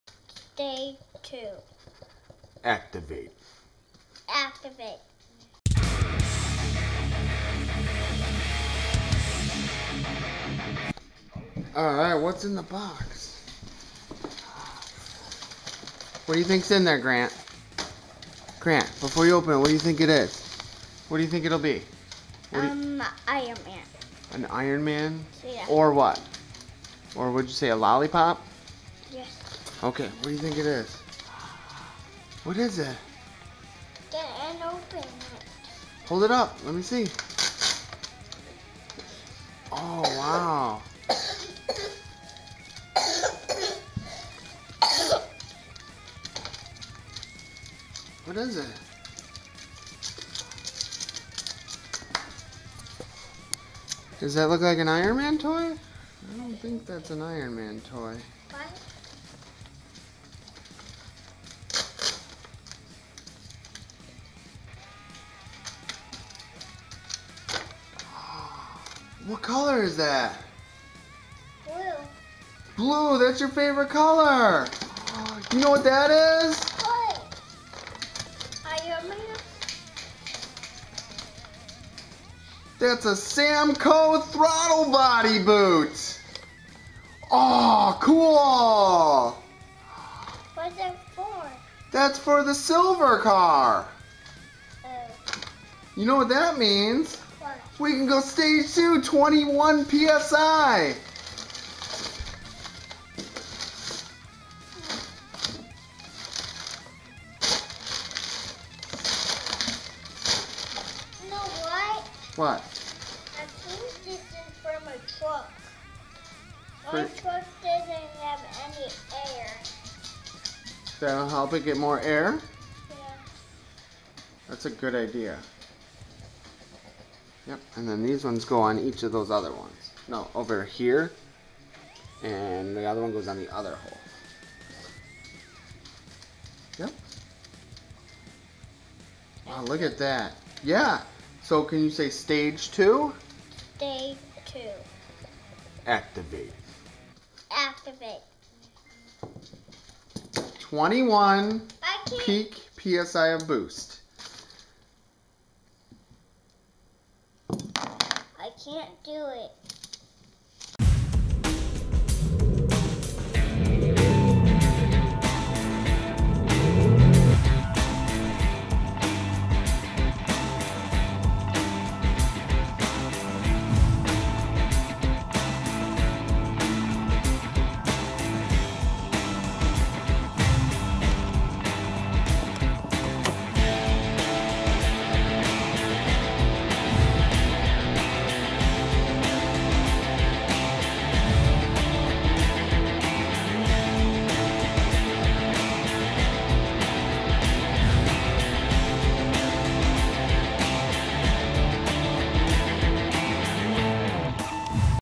2002 Audi S4: Ep. 50 - Samco Throttle Body Boot
A new package arrives. We pick up a cheap, used, throttle body boot! .